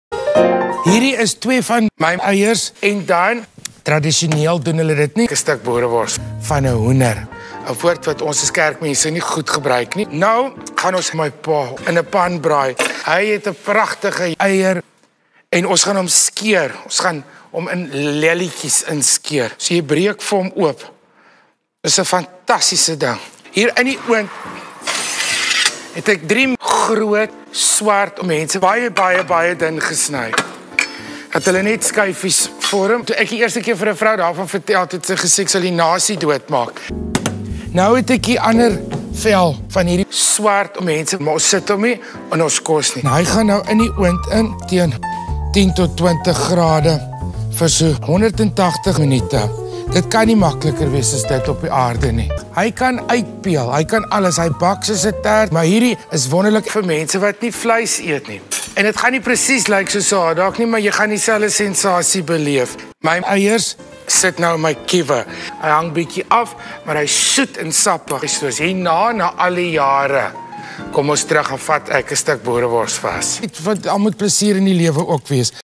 Nataniël kook sy pa en sy eiers
'n Parodie op Die Nataniël Tafel